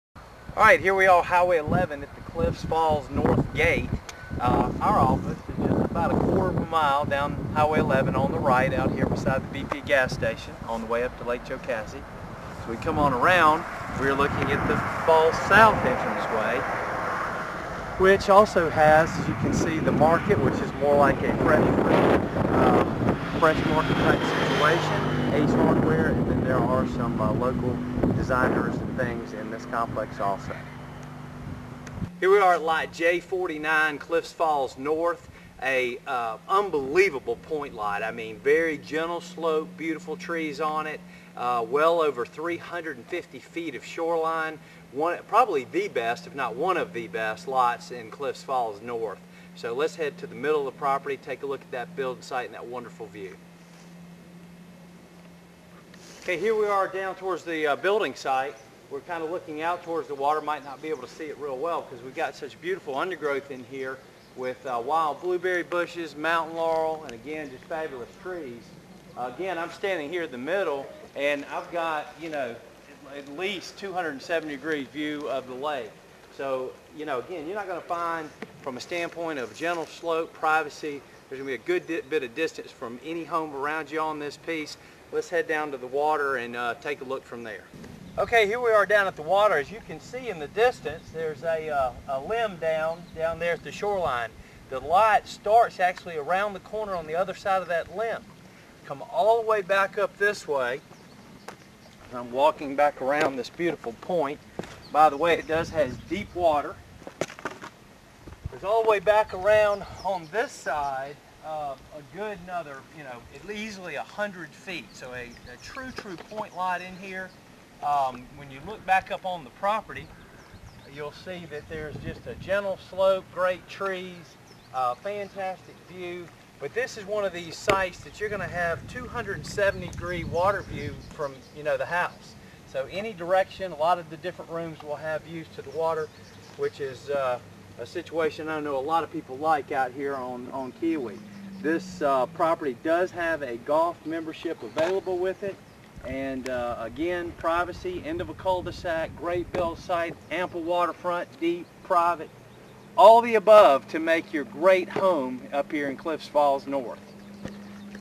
Lake Keowee Real Estate Lot J-49 Cliffs at Keowee Falls North Mike Roach
Excellent piece of Lake Keowee Real Estate located on the norht end of the lake.  This property is a point lot looking south down Lake Keowee.  Lake Jocassee is within 5 minutes of this property.  So you can easily enjoy both Lake Keowee and Jocassee as well as all of the excellent amenities The cliffs communities have to offer.